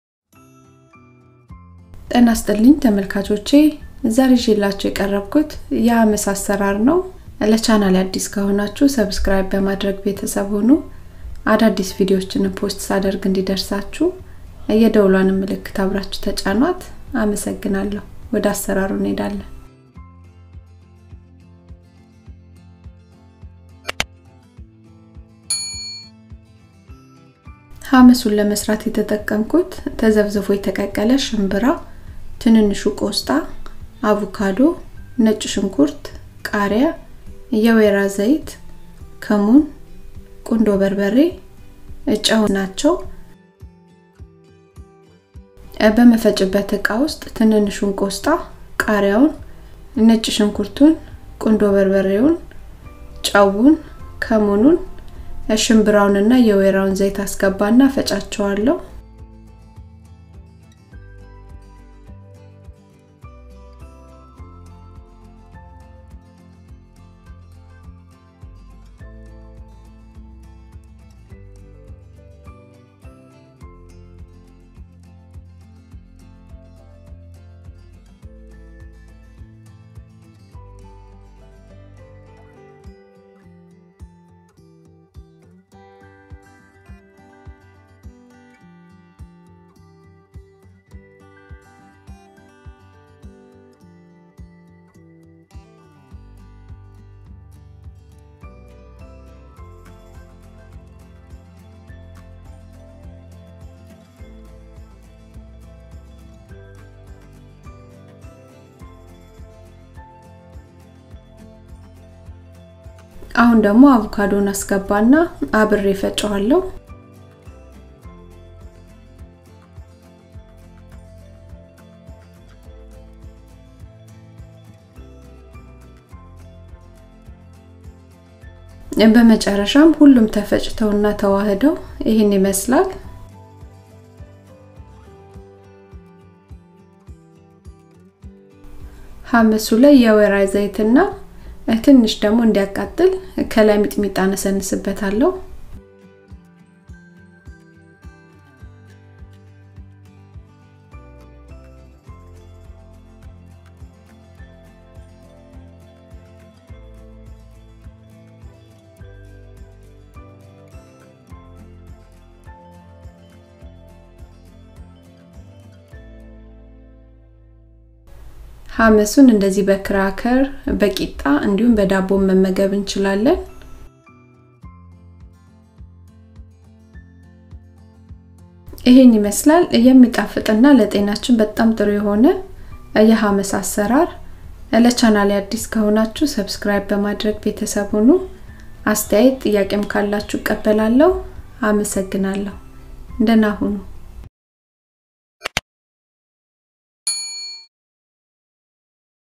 በሽንብራ በአቮካዶና በትንንሹ የቆስጣ ቅጠል የተሰራ ምርጥ የሃመሥ አሰራር - EthioTastyFood/Ethiopian food
ሃመሱን ለመስራት የተጠቀምኩት
* ተዘፍዝፎ የተቀቀለ ሽንብራ
* ትንንሹ የቆስጣ ቅጠል
* አቮካዶ
* ነጭ ሽንኩርት
* ቃርያ
* የወይራ ዘይት
* ከሙን
* ቁንዶ በርበሬ
* ጨው ናቸው
* ማባያውን እንድምርጫችን ዳቦ ቂጣ ወይም እንደኛ በክራከር መመገብ ይቻላል

EthioTastyFood
Ethio Tasty Food
ETHIOTASTYFOOD
ethiotastyfood
ETHIO TASTY FOOD
ethio tasty food